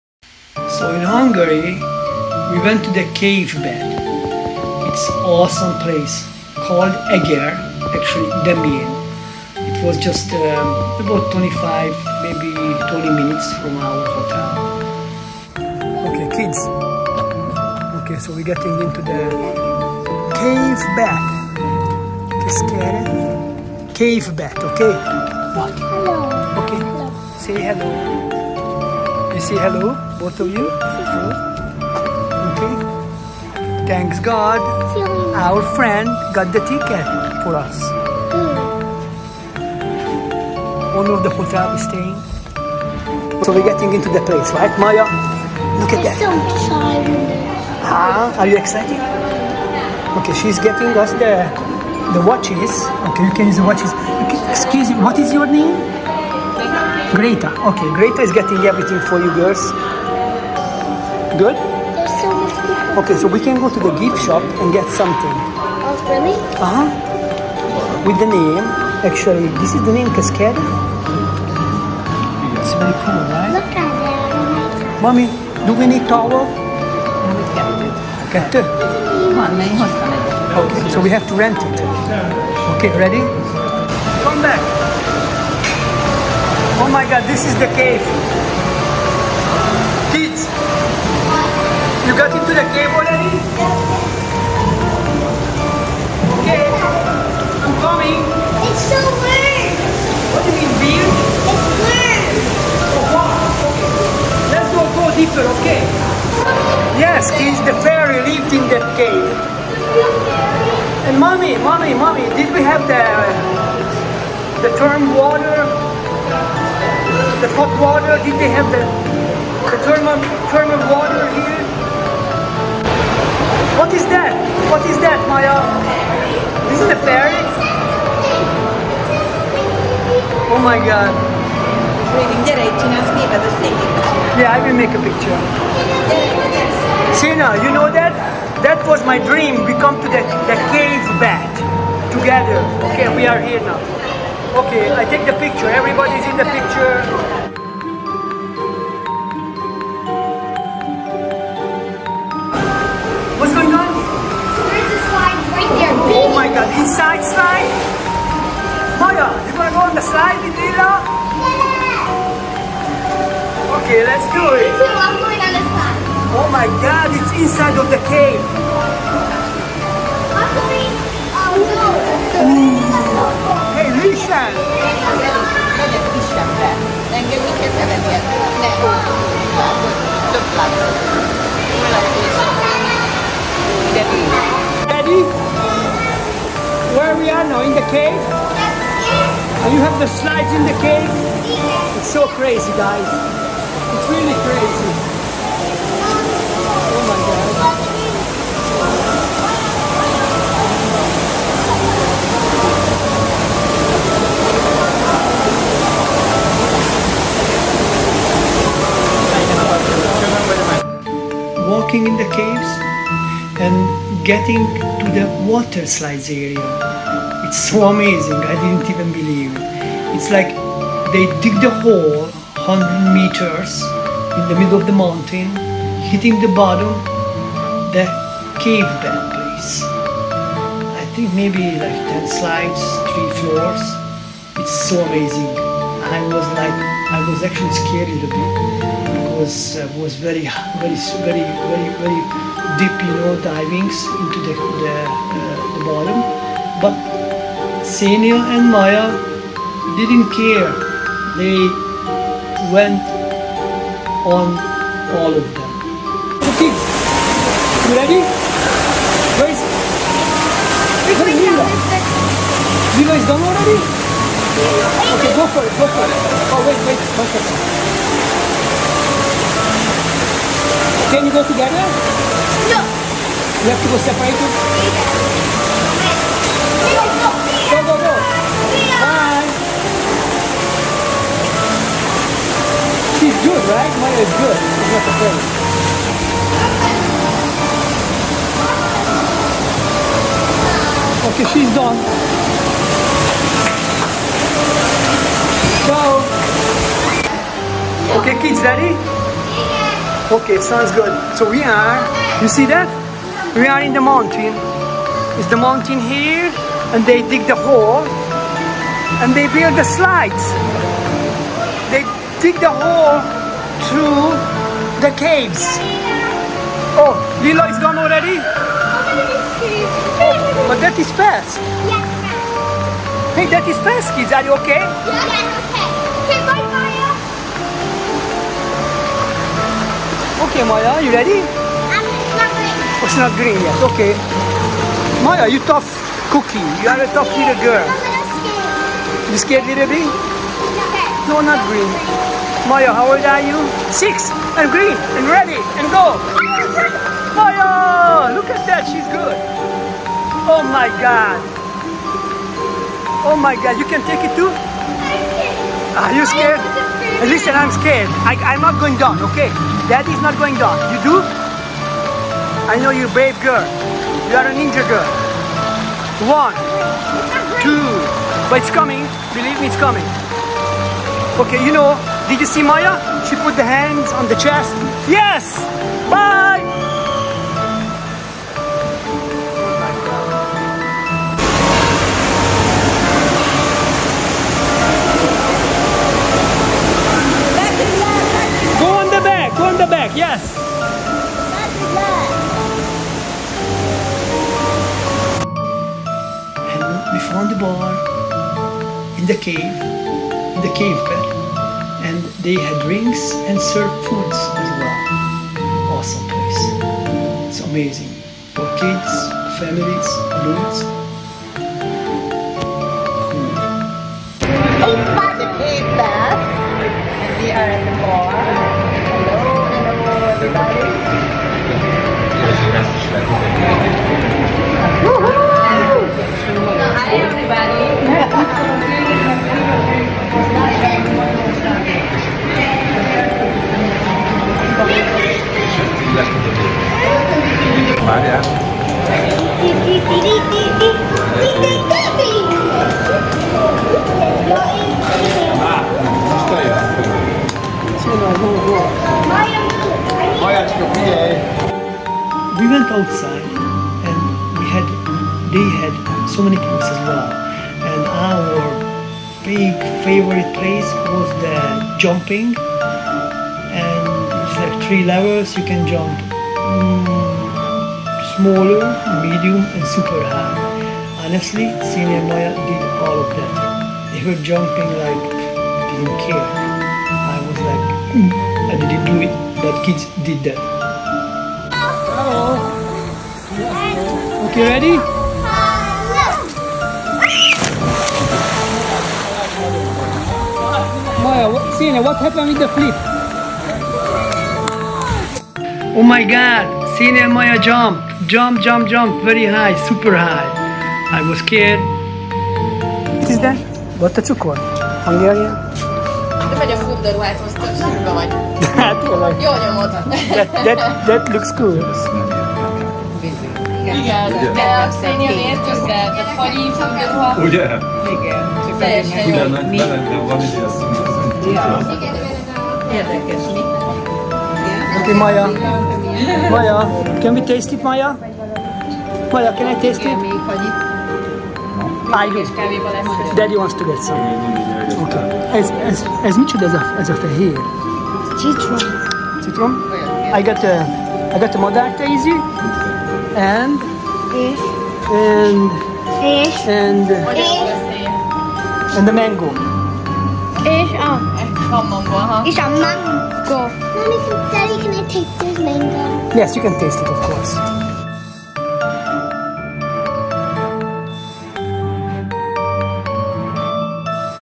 Cave Waterslides EU Vacation Season 1 Episode 5
🌟  Dangerous 100 Meters underground Cave slides😱💪Sisi Kids Adventure Alert! 🌟
Join Xenia and Maya as they explore the Hungarian Cascade Cave Bath! 🏞️💦 Dive into a magical world inside the mountain 🌄, where they take on 10 dangerous underground water slides 🌊🎢, plunging 100 meters deep into the earth! 😱💪 Watch them race, laugh, and discover the thrill of this epic adventure! 🚀 Don't miss it! 🎉